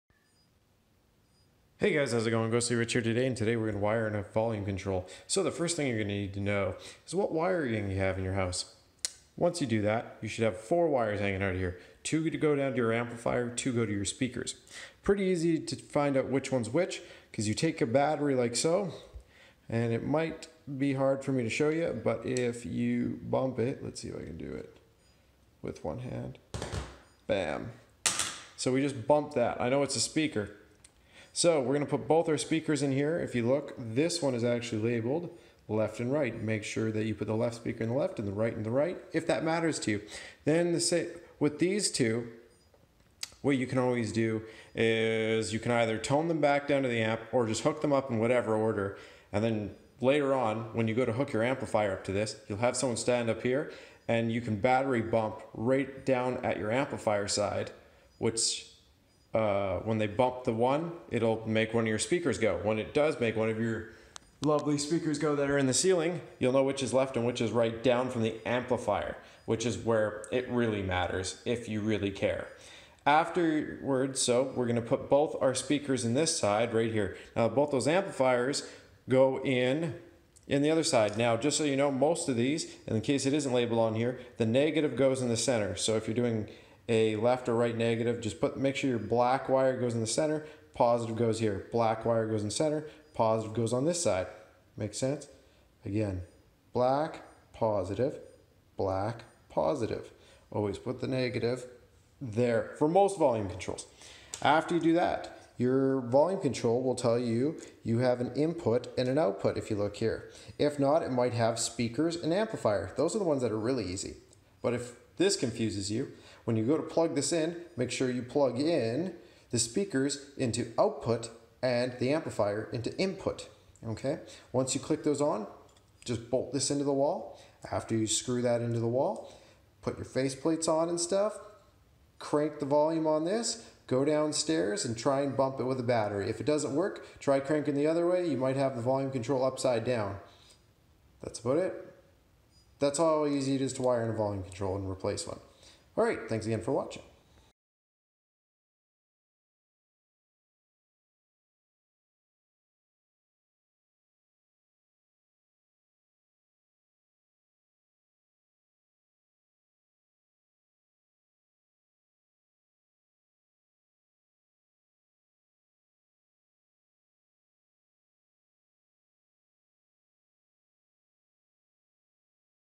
HOW TO INSTALL AND WIRE A VOLUME CONTROL
This is just a quick tutorial on how to  wire and install a volume control in your house, this should help you also change out or replace a volume control as well. thanks again for watching and i hope this helps you out with your diy project.